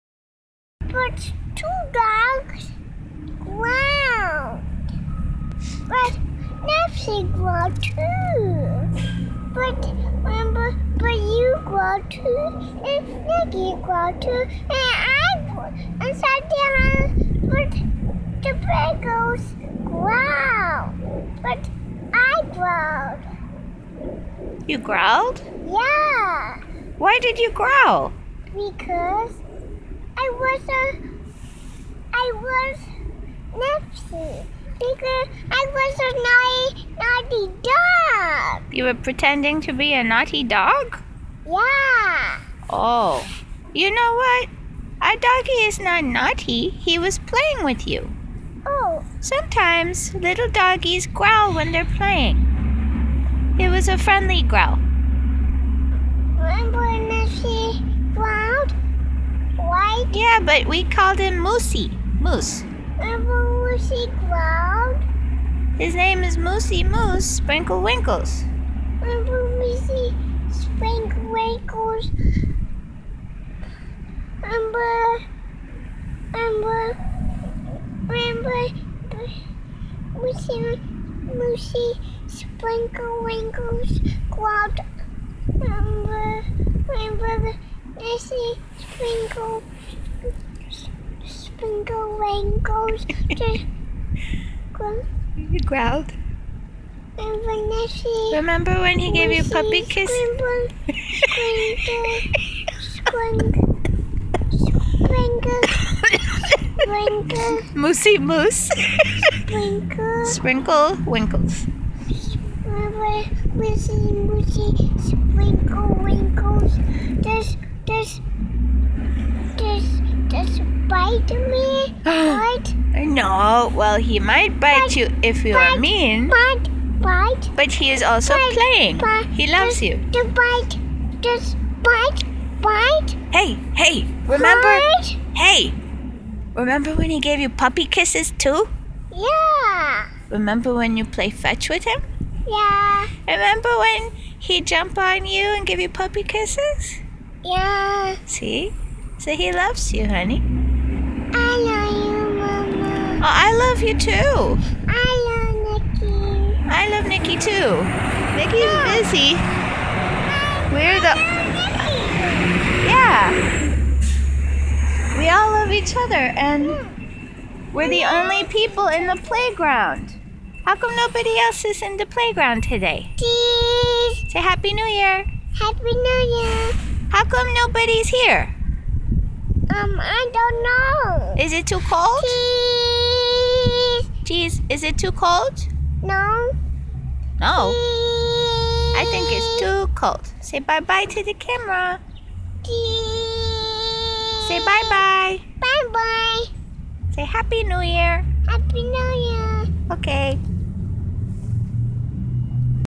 2 year old dog whisperer
My toddler discussing dog behavior, how to understand dogs, and dog aggression. Also trying to say our puppy's name, Moosi Mousse Sprinkle Winkles.